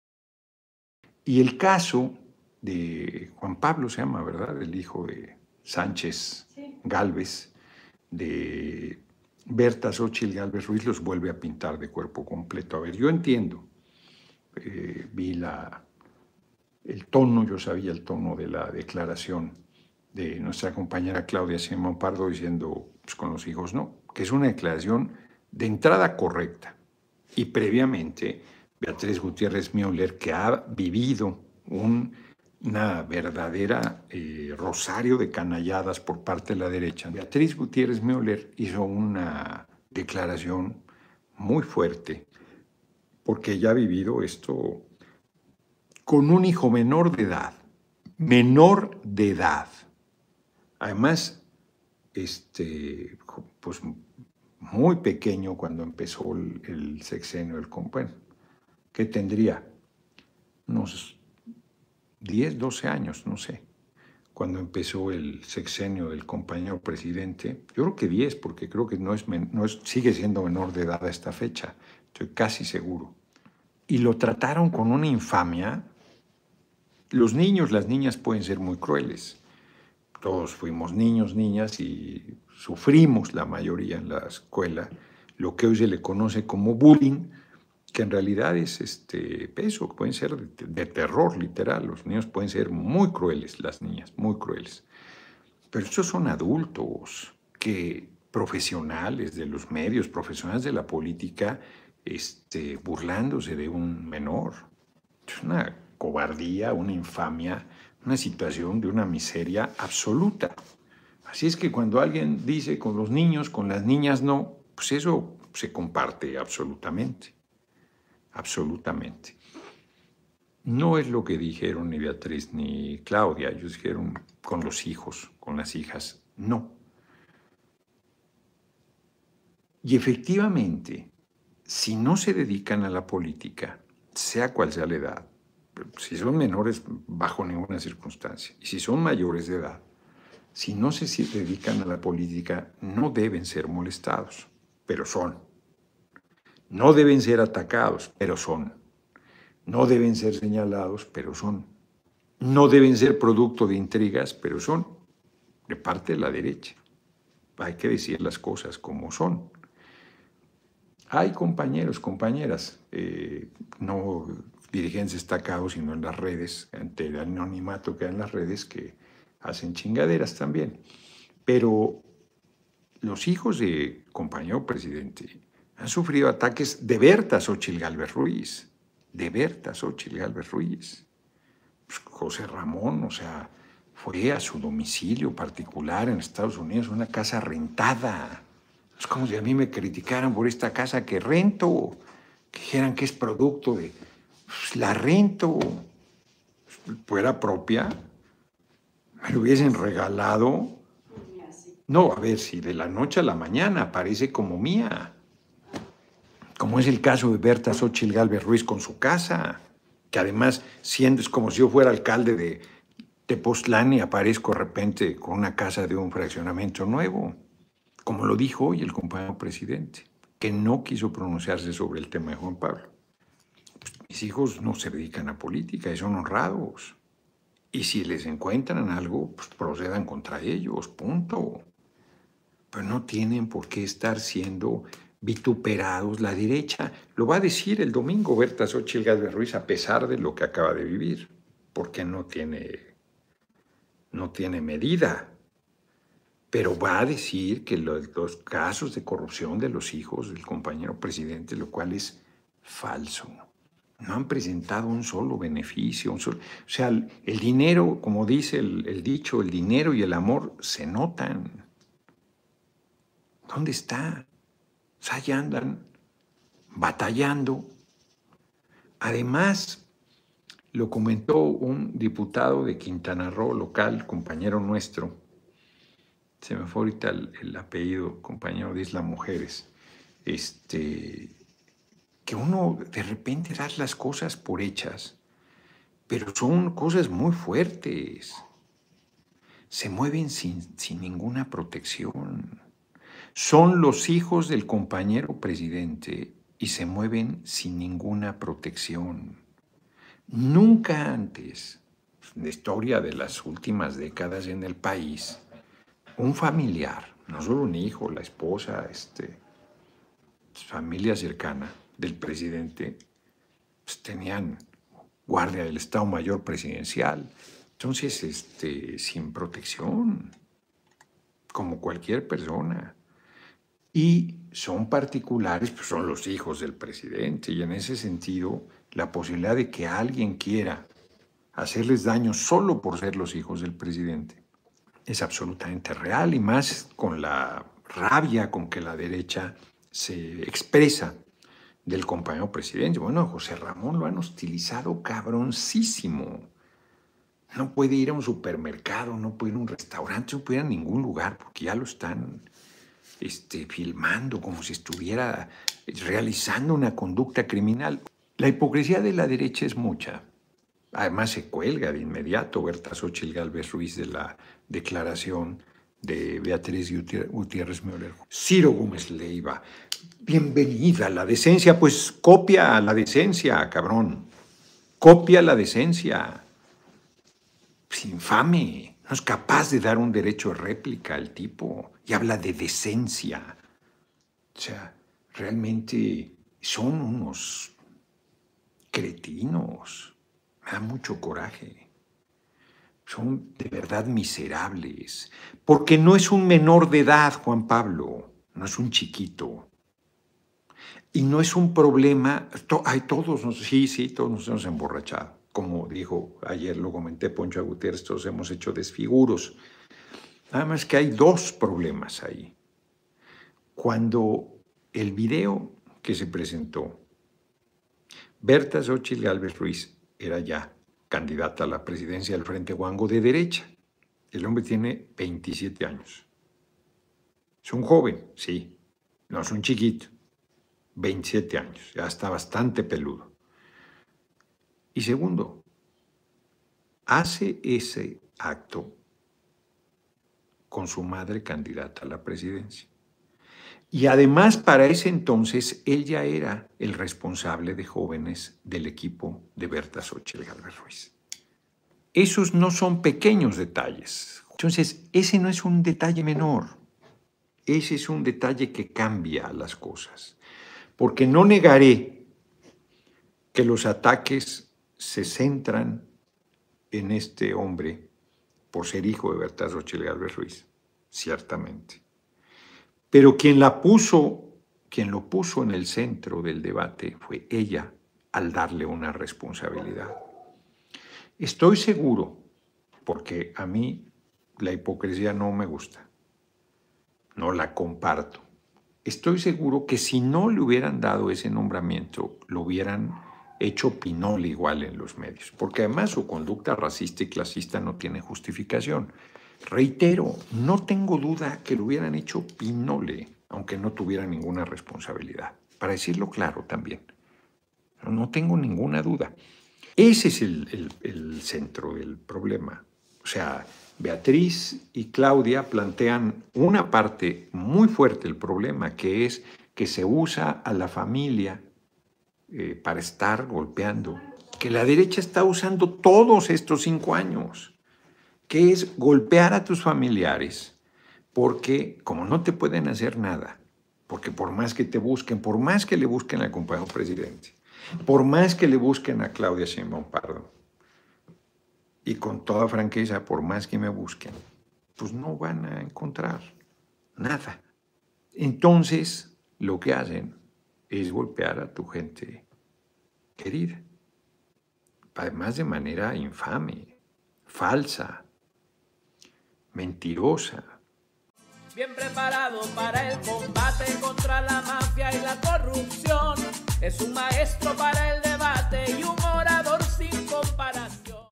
El Hijo de Xóchitl y los profesionales de la política burlándose de un niño —SinAparenteDigresión
Muchas gracias por participar con nosotros compartiendo los mensajes del Lic.

Fragmento de la videocharla del Diputado y coordinador de enlace con organizaciones sociales y civiles de la Dra. Claudia Sheinbaum Pardo, Gerardo Fernández Noroña correspondiente al día 4 de abril 2024, puedes ver nuestro resumen